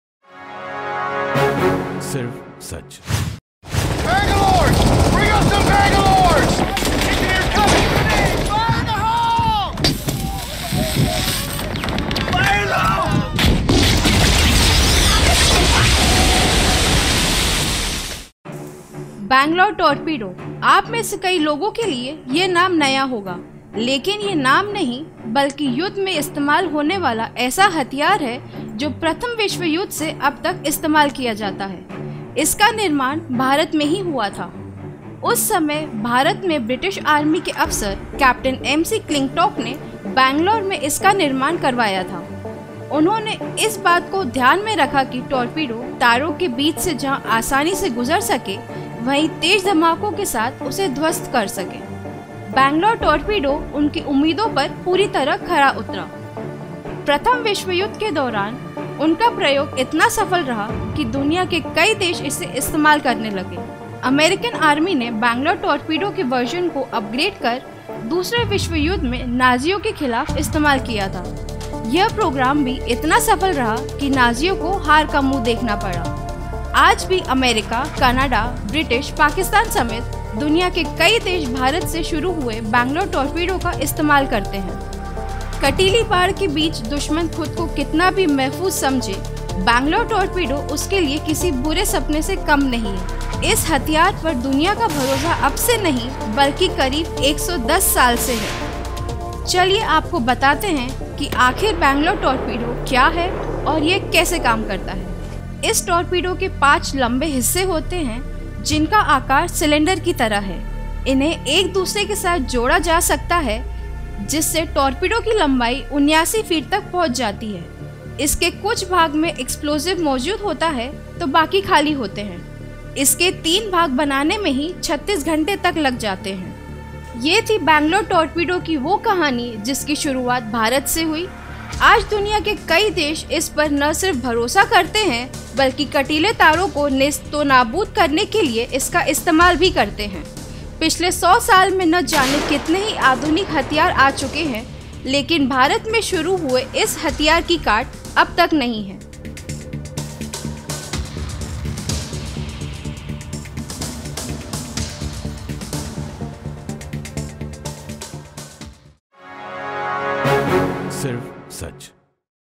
Bangalore Torpedo | India's Oldest weapon | Most famous weapon of World WARS | बैंगलोर टॉरपीडो
कटीली बाढ़ के बीच दुश्मन खुद को कितना भी महफूज समझे बैंगलौर टॉरपीडो उसके लिए किसी बुरे सपने से कम नहीं। इस हथियार पर दुनिया का भरोसा अब से नहीं बल्कि करीब 110 सालों से है। चलिए आपको बताते हैं कि आखिरकार बैंगलौर टॉरपीडो क्या है और यह कैसे काम करता है।